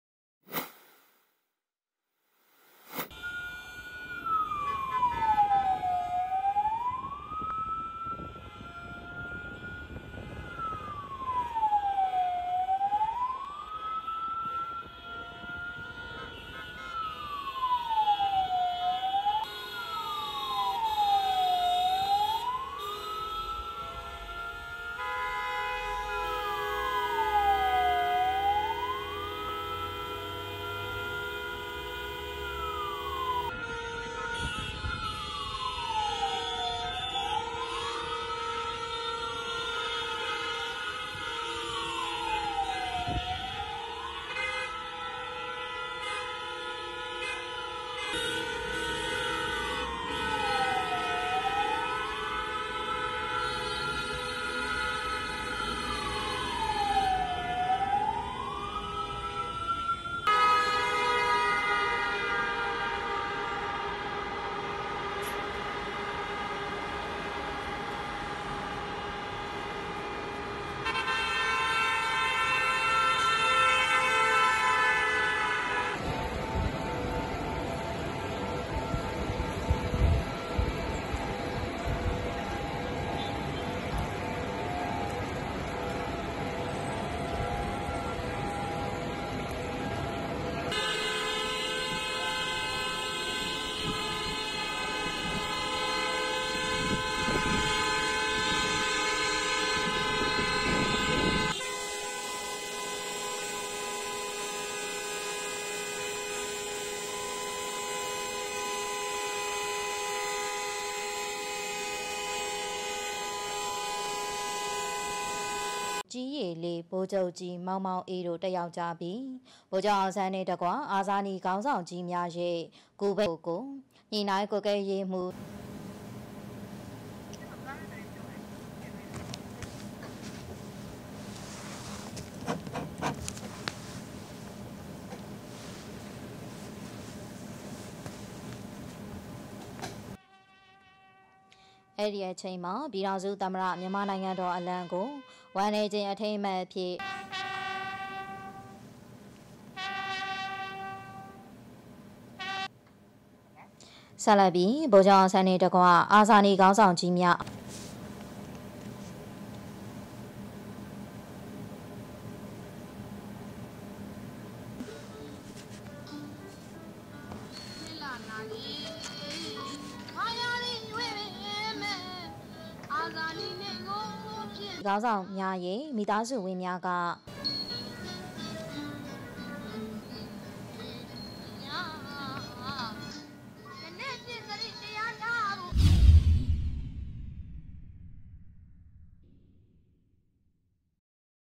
Honking cars mark 75th anniversary of killing of Myanmar's independence hero
Myanmar marks the 75th anniversary of the assassination of its independence hero Aung San with drivers on Yangon's streets honking against junta rule. Aung San Suu Kyi, the daughter of the man known affectionately as "Bogyoke" (General), is currently detained by the military her father helped create.

Parallelly, state media footage shows the junta's number two Soe Win placing a wreath at the Martyrs' Mausoleum in Yangon -- another spot usually bustling with those wishing to pay their respects, but which on Tuesday was limited to invitees only.